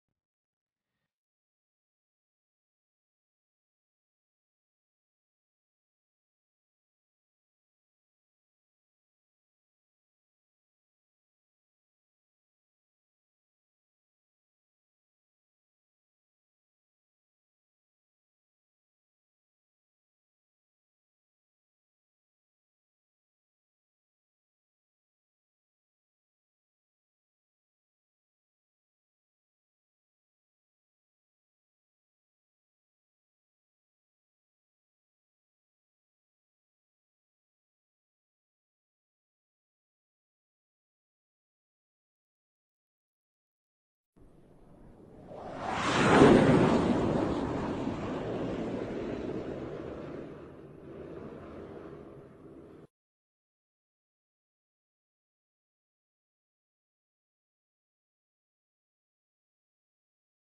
How satellite places in the Eearth orbit: Upagrah keise sansthapit kiya jata hey!
How satellite places in the earth orbit
learn the salellite placing technic in the earth orbit

satellite prithbee ki kaksha me keise sansthapit kiya jata he detail me janiye